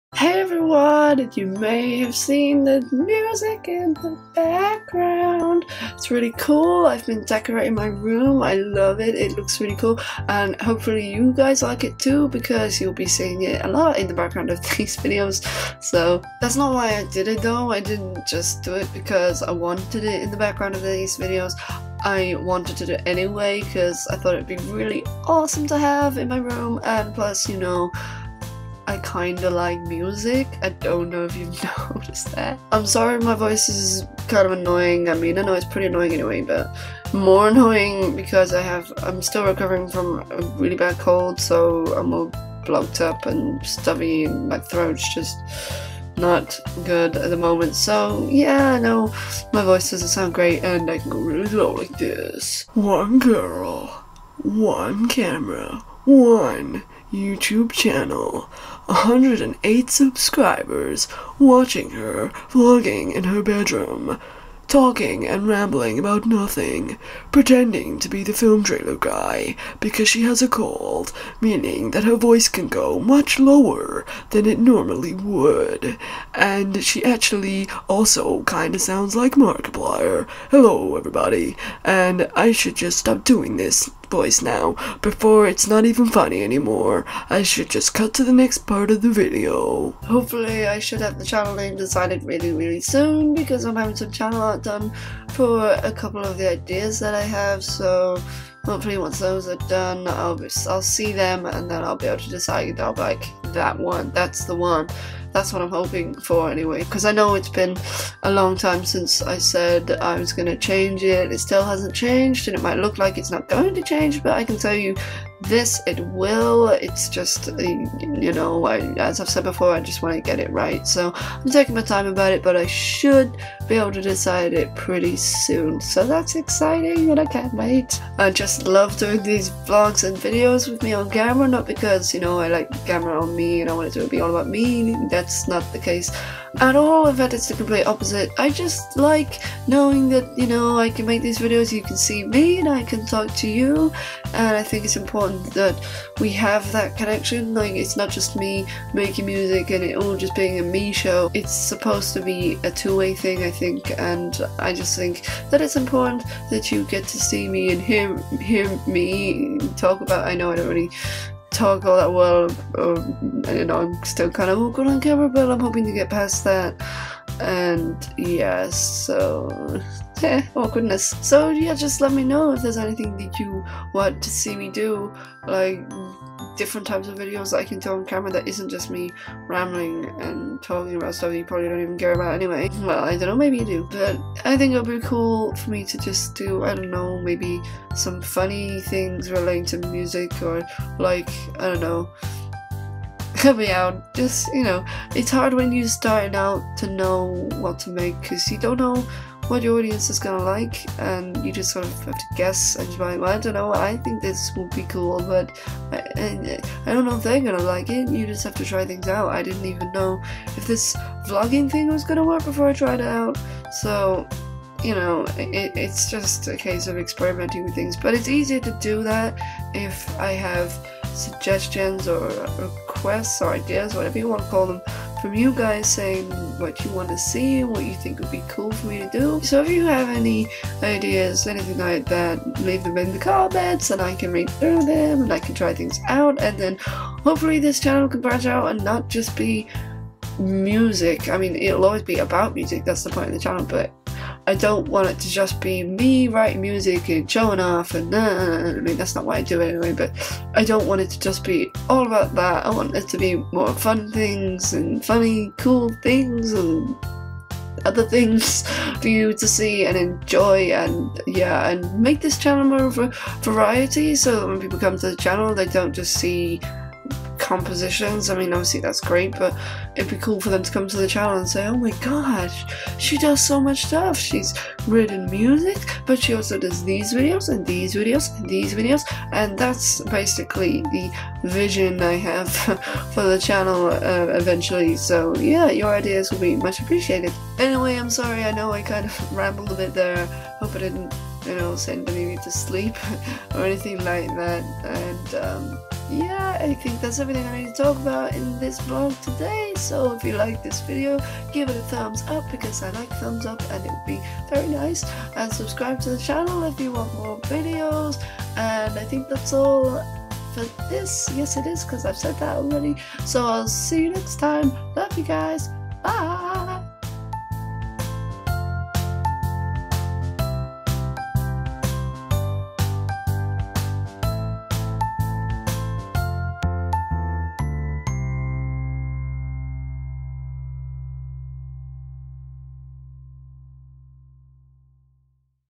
MUSIC ON THE WALL! | Vlog #7
Talking about some cool stuff that has been happening, the channel, and talking in a silly voice because of having a cold :P

The video was made using QuickTime (to record), iMovie (to edit) and the thumbnail was made using PhotoScape X.